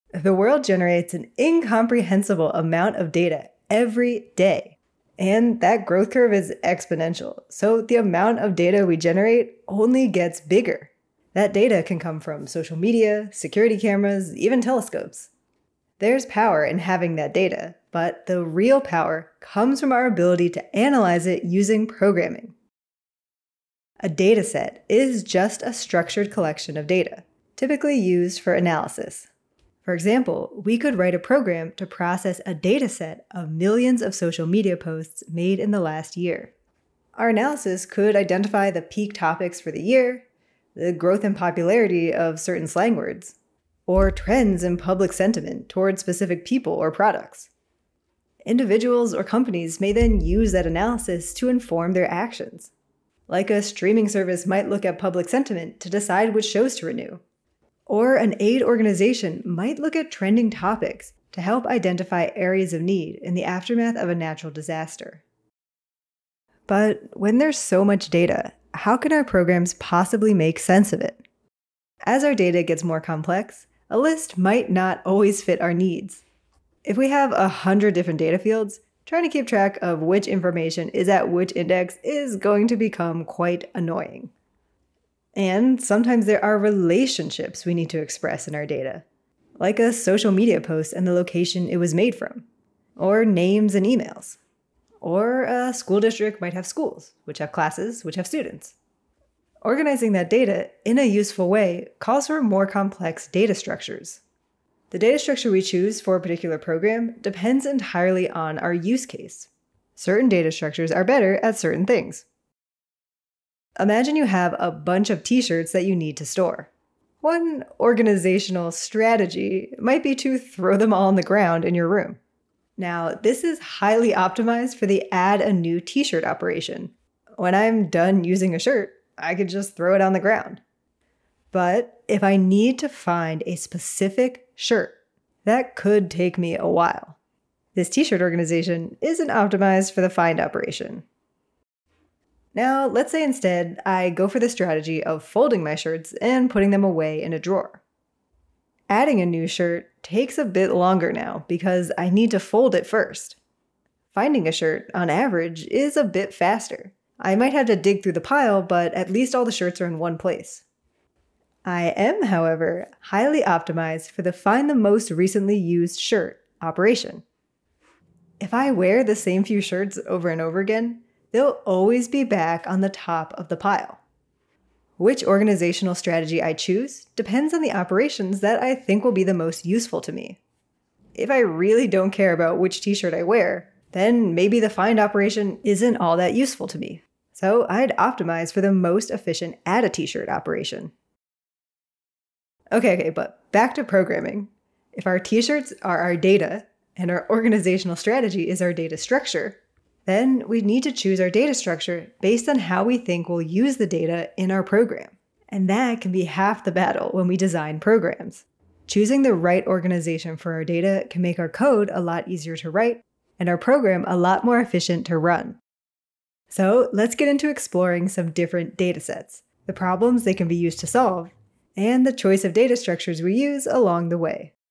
Datasets and data structures | Intro to CS - Python | Khan Academy
How can you use programming to make sense of large amounts of data? Learn how organizations use data insights to guide real world actions. Consider the tradeoffs between different data structures based on your use case. How does the way your organize your data impact your program?

TIMESTAMPS:
00:00 : the world is full of data!
00:24 : data insights - social media dataset
01:12 : why do we need data structures?
01:53 : organizing t-shirts - on the ground
02:22 : organizing t-shirts - folded
02:55 : organizing t-shirts - tradeoffs
03:12 : choosing a data structure